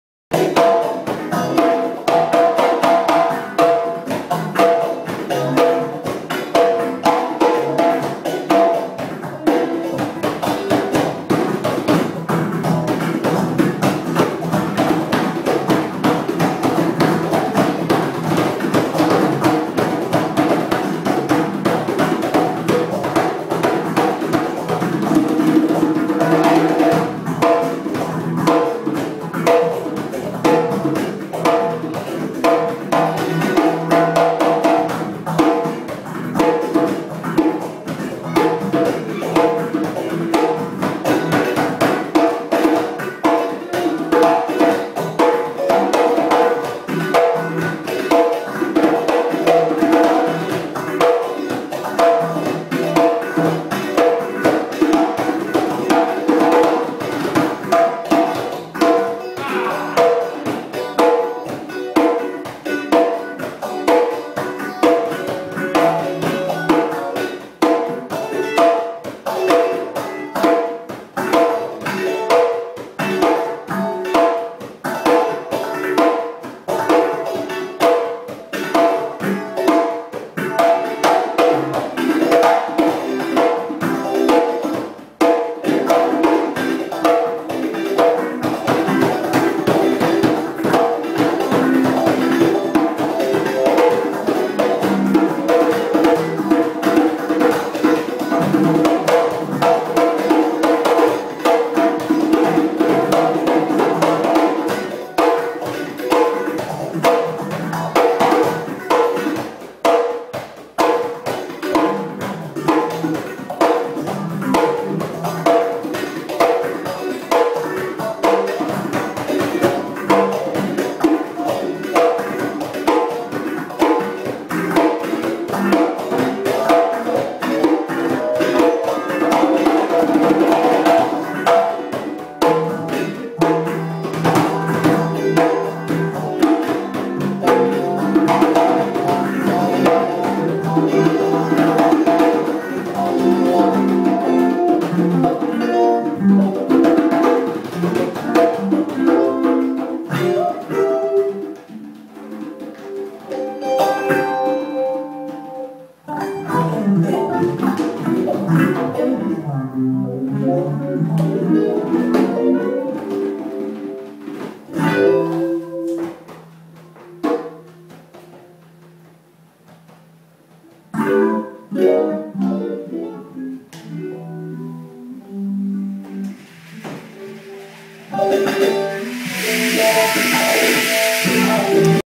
Kiyoshi Fujikawa×Seiji Takeuchi＠Spoon Bread ②
2011年7月3日、日曜日の自由が丘・Sweet Forest 2階のカフェ●スプーンブレッド。
ジャンベ・藤川清さんとギタリスト・Seijiさんのセッション！